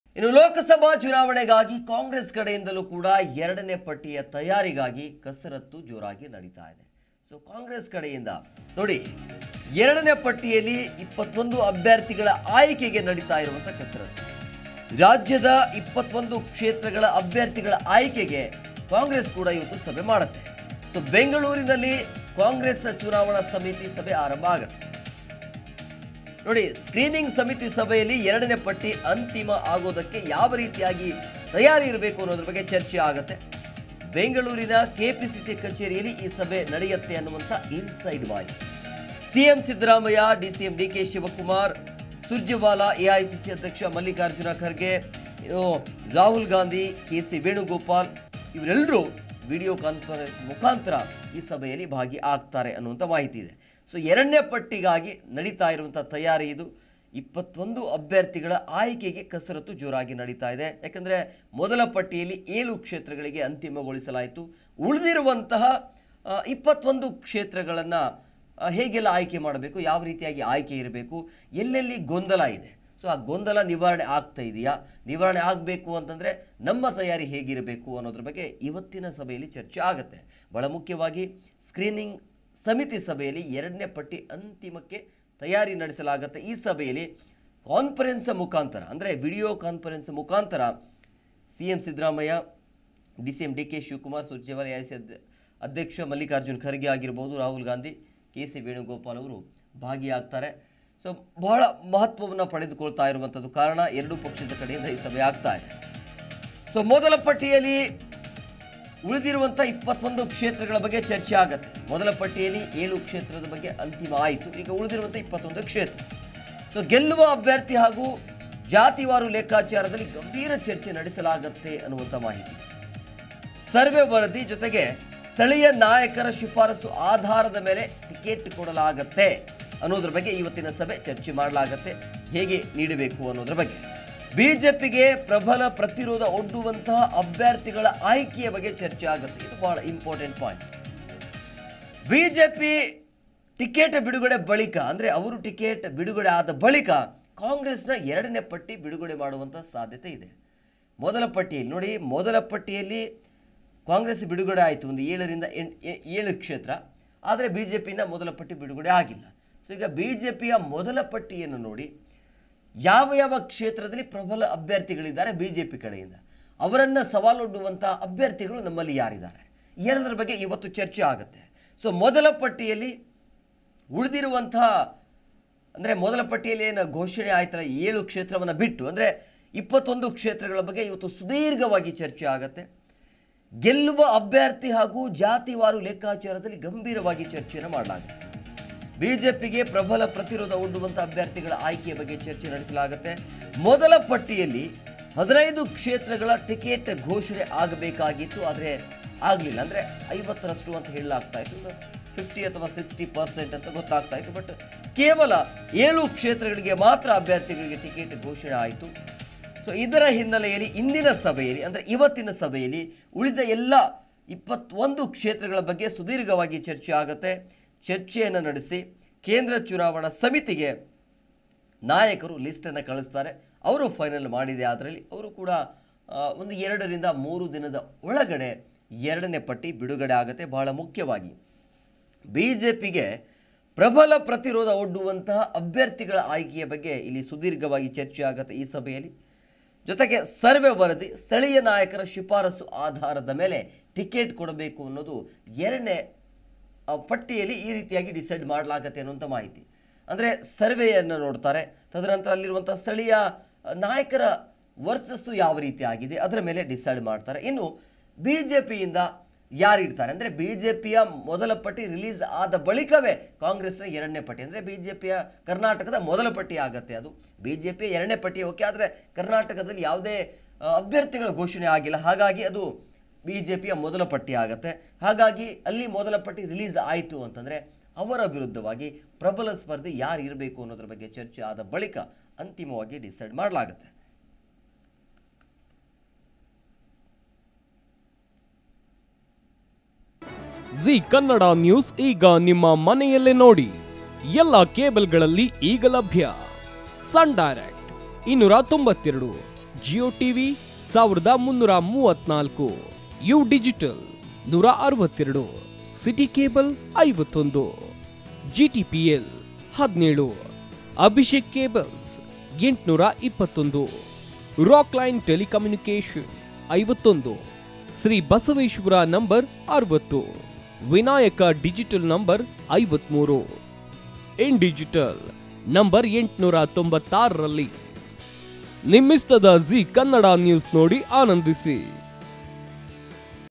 BNG CONG FILE+KHARGE+SUJBangalore | ರಾಜ್ಯದ 21 ಕ್ಷೇತ್ರಗಳ ಅಭ್ಯರ್ಥಿಗಳ ಆಯ್ಕೆಗೆ ಕಾಂಗ್ರೆಸ್‌ ಸಭೆEWALA FILE
ಸಿಎಂ,ಸಿದ್ದರಾಮಯ್ಯ,ಡಿಸಿಎಂ ಡಿ ಕೆ ಶಿವಕುಮಾರ್, ಸುರ್ಜೇವಾಲ,ಎಐಸಿಸಿ ಅಧ್ಯಕ್ಷ ಮಲ್ಲಿಕಾರ್ಜುನ ಖರ್ಗೆ,
ರಾಹುಲ್ ಗಾಂಧಿ, ಕೆ.ಸಿ.ವೇಣುಗೋಪಾಲ್ ವಿಡಿಯೋ ಕಾನ್ಫರೆನ್ಸ್ ಮೂಲಕ ಸಭೆಯಲ್ಲಿ ಭಾಗಿ